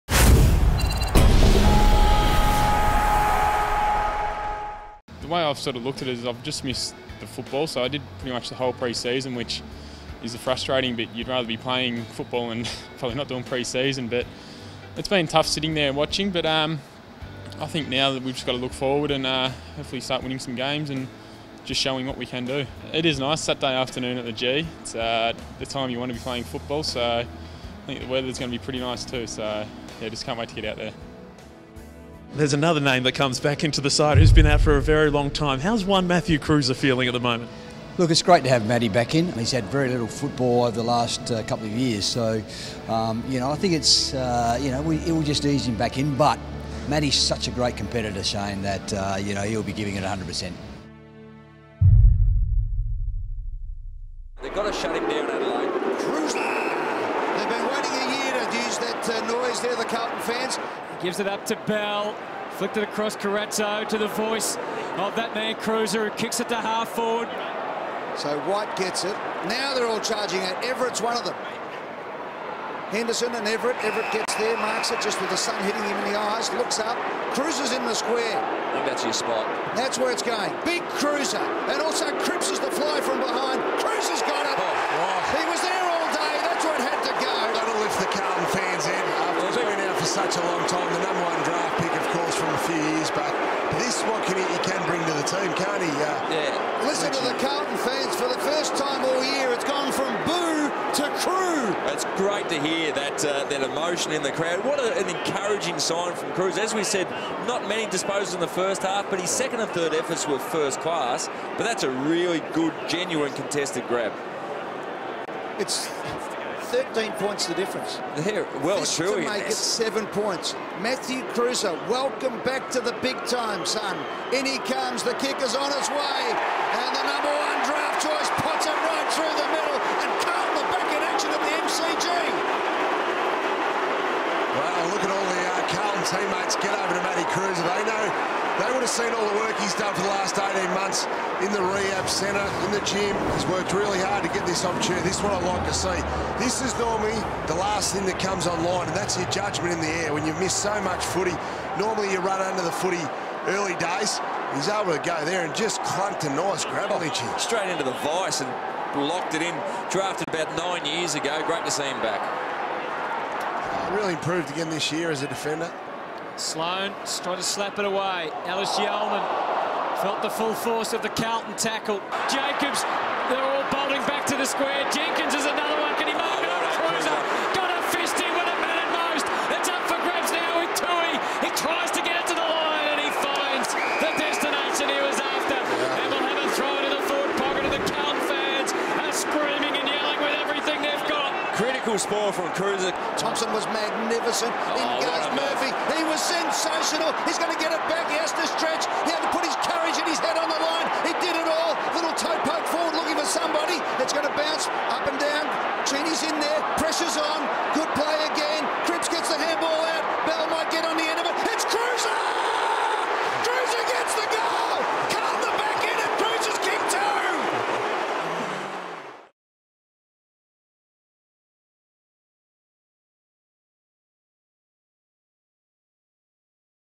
Kreuzer's big return
Watch the story of Matthew Kreuzer's return to AFL from start to finish.